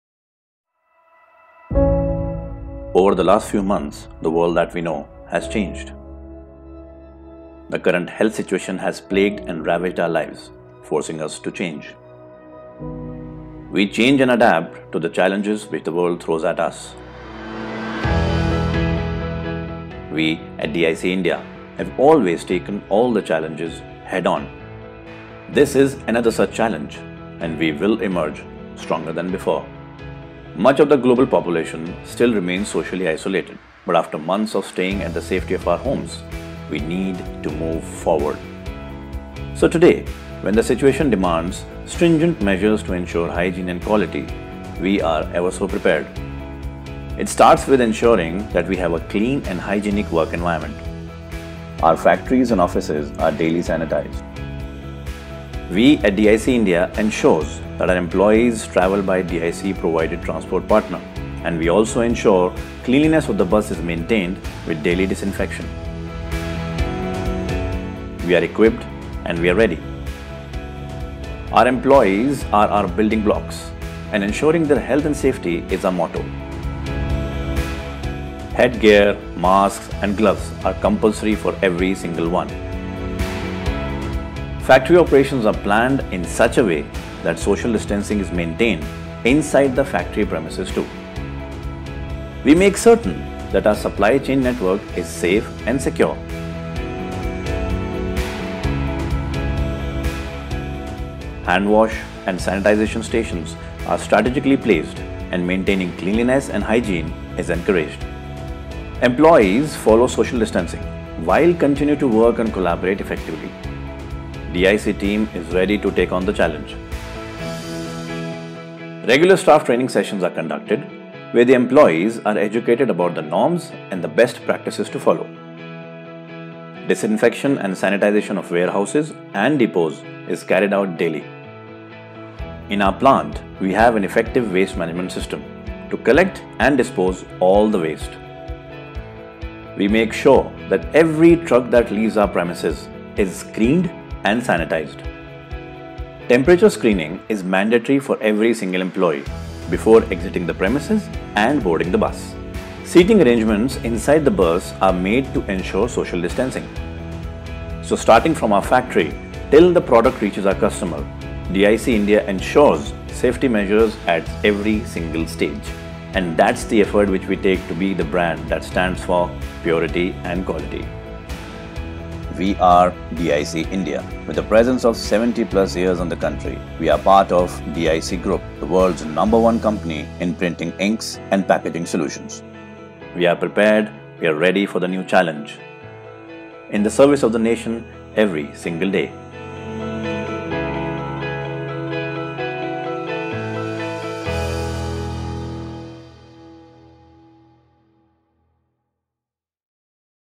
DIC Factories and Offices Sanitisation
We are at DIC India take immense pride in the Hygiene protocols that we follow and safety protocols the employees follow  in the current health crisis across the world.